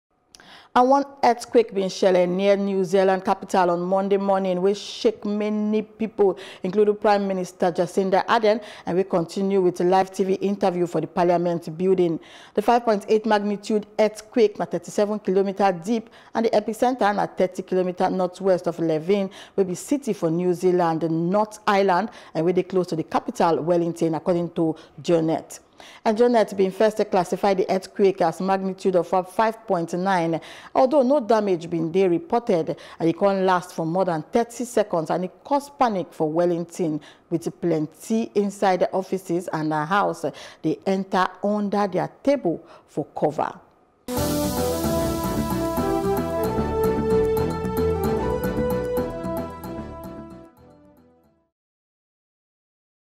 Earthquake For New Zealand: Earthquake Do Badness For New Zealand's Capital - Pm
One earthquake shele near New Zealand's capital on Monday morning, wey shake many residents including Prime Minister Jacinda Ardern wey continued with a live TV interview for the parliament building.